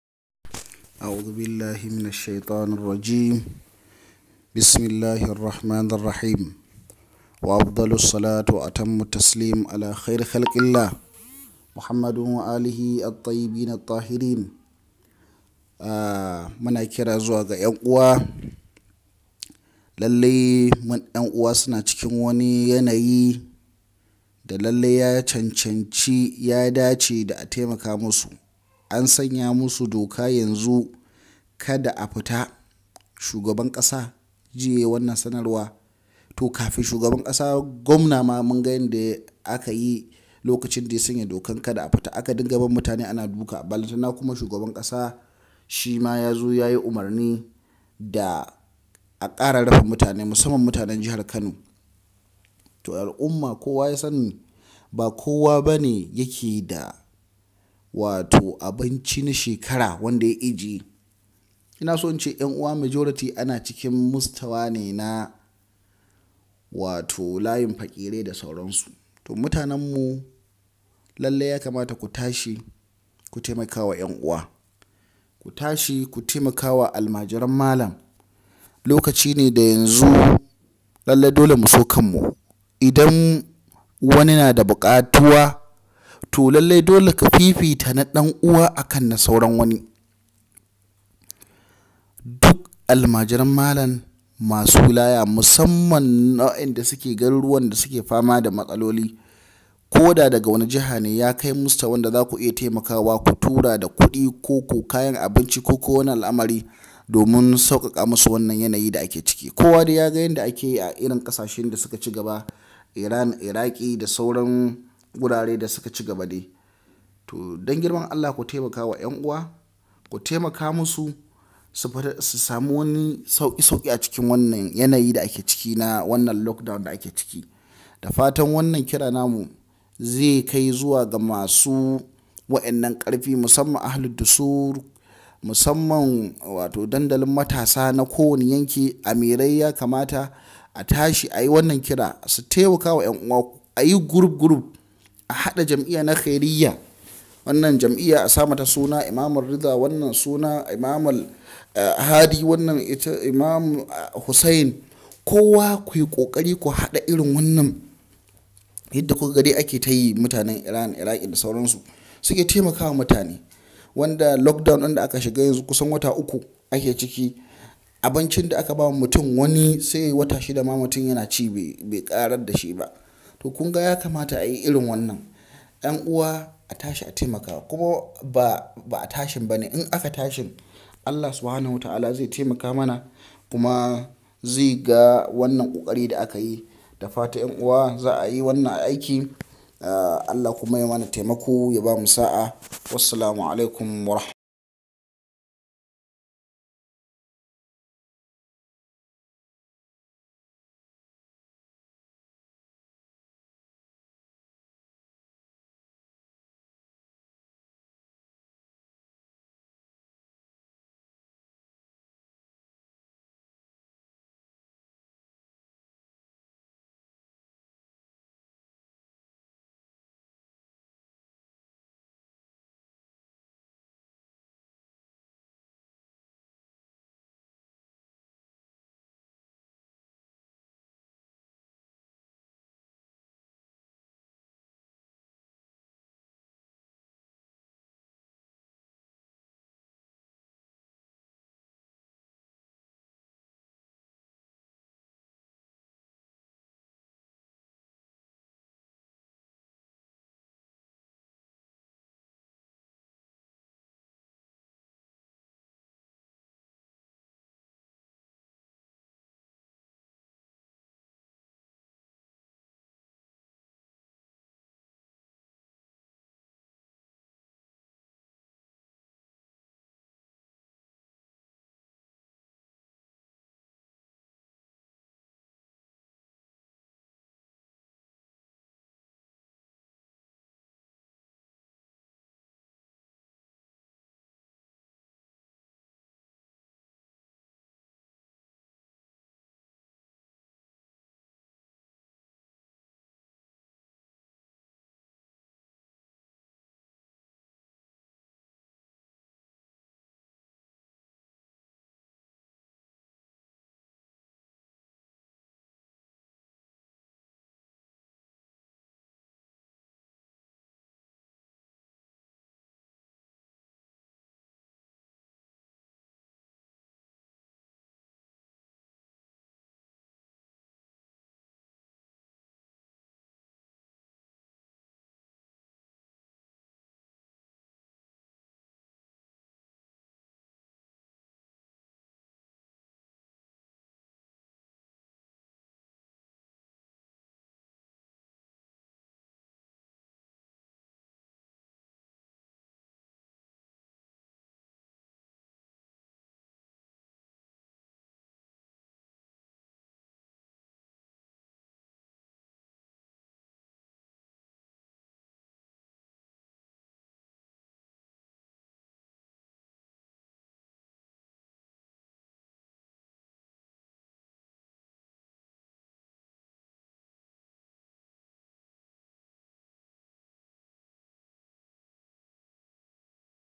Kira zuwa ga Musulunci da sutashi lallai su Taimaki Ƴan uwansu
Wannan Kirane Zuwa ga Almajiran malam da lallai su tashi su Taimaki Ƴan uwansu musamman a wannan lokaci na Lock down ɗin